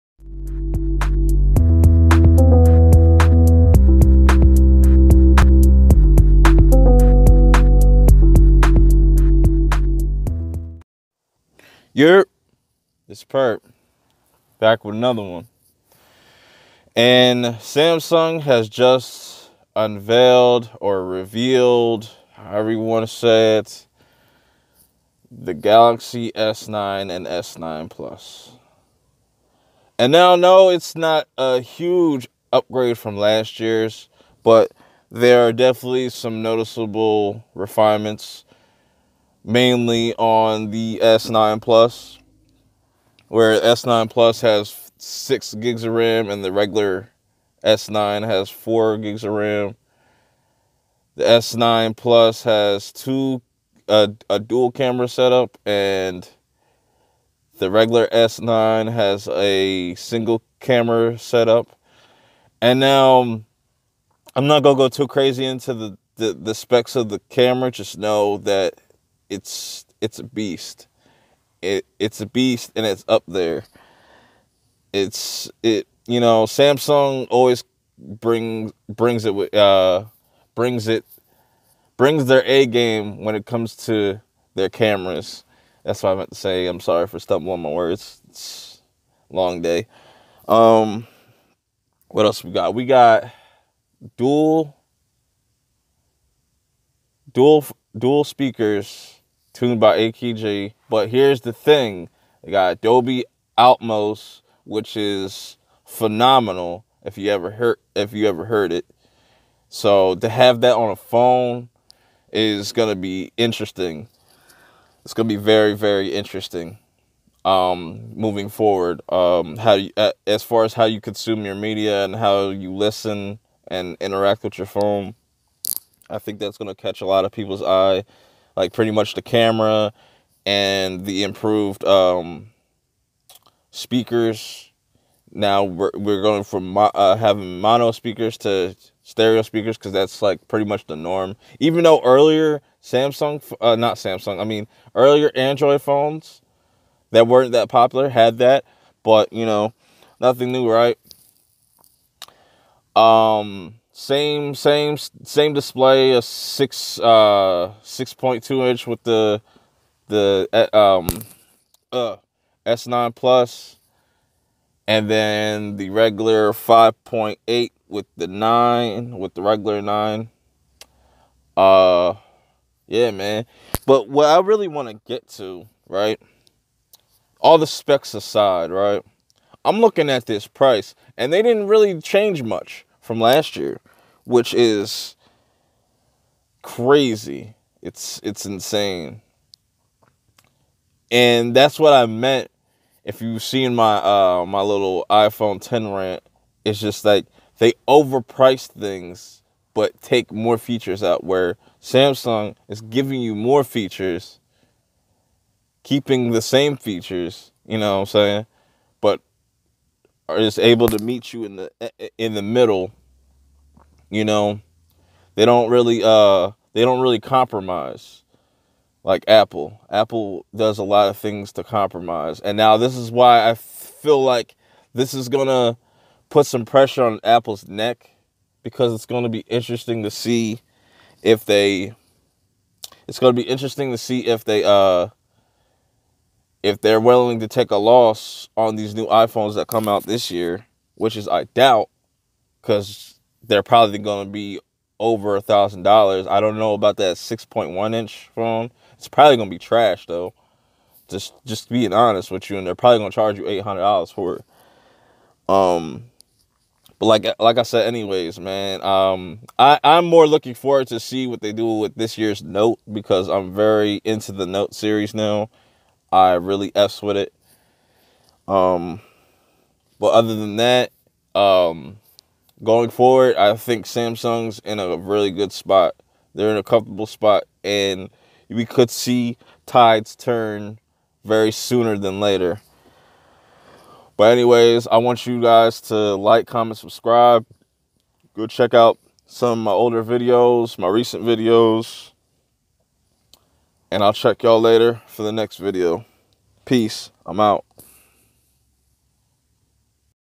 Samsung Galaxy S9 and S9 PLUS Reveal At Samsung Unpacked 2018 My Thoughts
Galaxy S9 S9PLUS